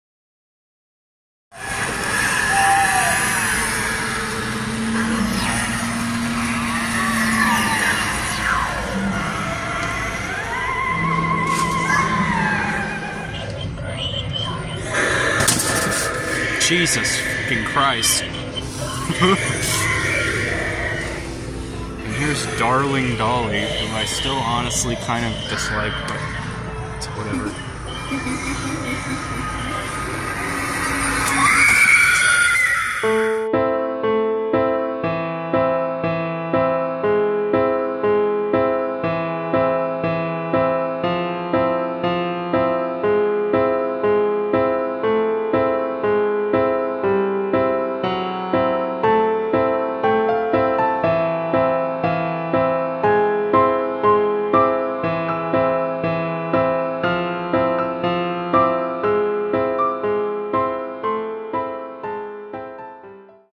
🌟 [Sardonyx] Vlogs - New Props And Theme At Kingsport, Spirit Location! 🌟
Brief scope of the Kingsport, TN Spirit location!